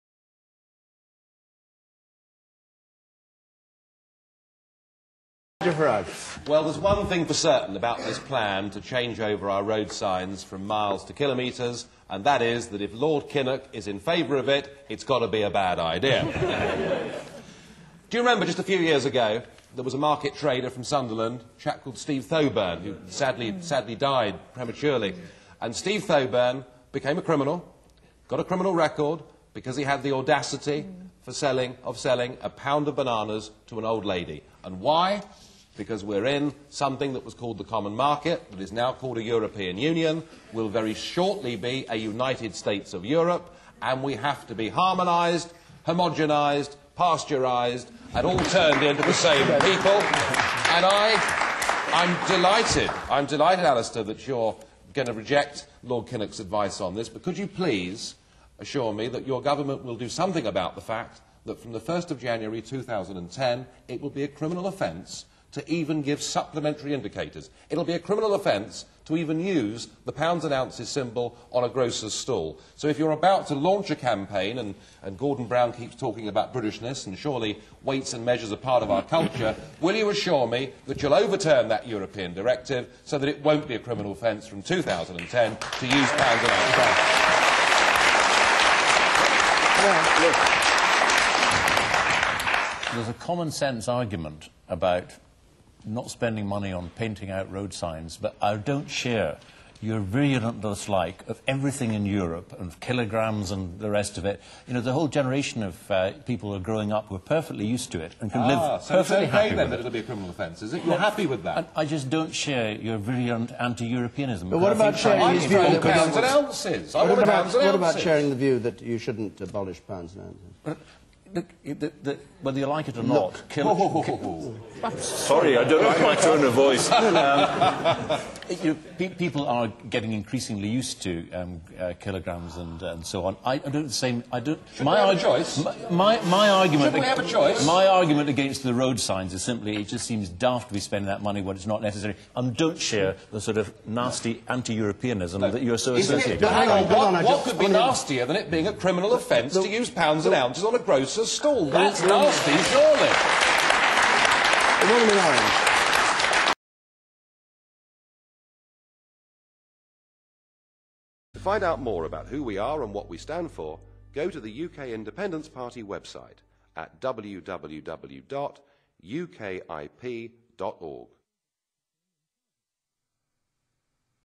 UKIP Nigel Farage and Labour Alistair Darling heated exchange over UK measurements
Heated undertones as UKIP Nigel Farage and Labour Alistair Darling debate Neil Kinnock's desire for UK road measurments to change from miles to KM.